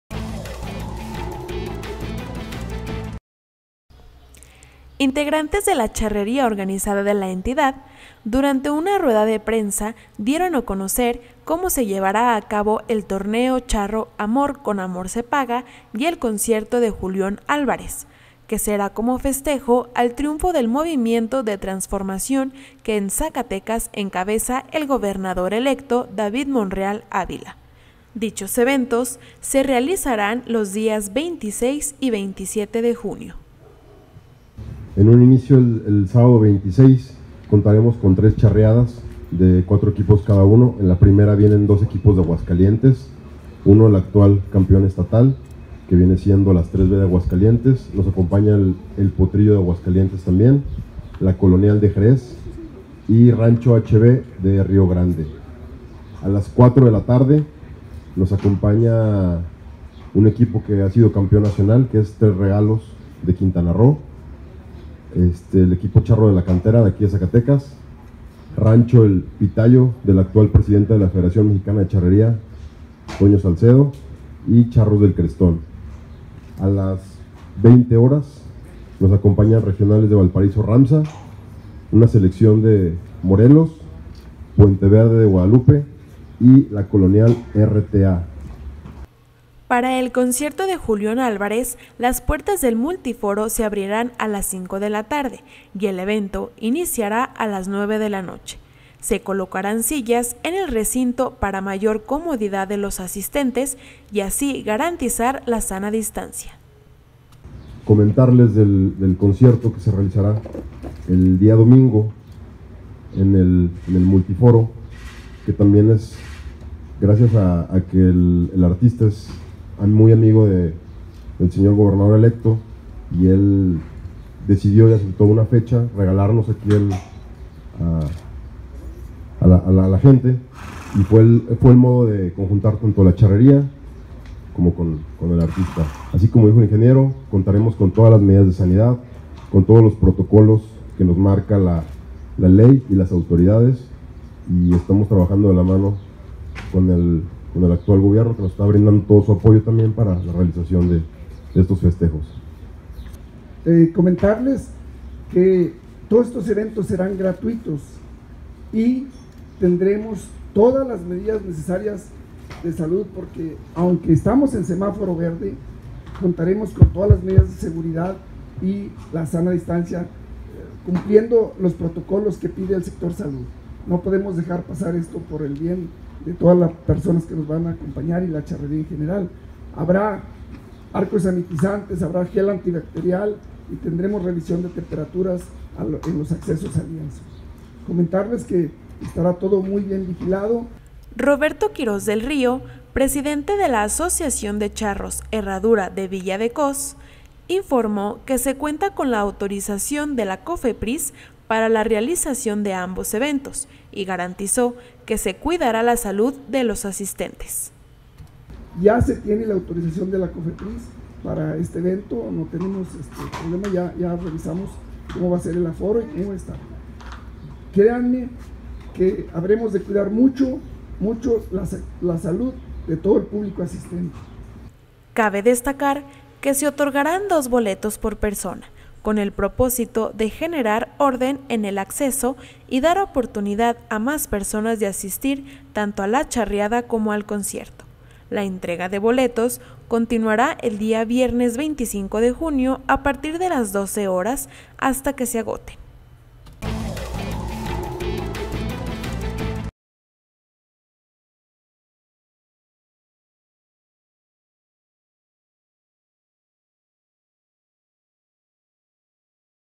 7 Charros de Zacatecas presentan detalles de “Amor con amor se paga”
Los días 26 y 27 de junio se llevarán a cabo el torneo charro Amor con Amor se Paga y el concierto de Julión Álvarez para festejar el triunfo del movimiento de transformación que en Zacatecas encabeza el gobernador electo, David Monreal Ávila, y los boletos ya están disponibles, de manera gratuita, en las taquillas del Monumental Lienzo Charro Antonio Aguilar. Cabe destacar que se otorgarán dos boletos por persona con el propósito de generar orden en el acceso y dar oportunidad a más personas de asistir tanto a la charreada como al concierto. La entrega de boletos continuará este viernes 25 de junio, a partir de las 12 horas y hasta que se agoten. Roberto Quiroz del Río, presidente de la Asociación de Charros Herradura de Villa de Cos, informó que se cuenta con la autorización de la Cofepris para la realización de ambos eventos y garantizó que se cuidará la salud de los asistentes. Por tanto, aunque Zacatecas está en semáforo verde, se cumplirán los protocolos establecidos por el sector sanitario, entre ellas arcos sanitizantes, gel antibacterial, medición de temperatura en los accesos y vigilancia de la sana distancia.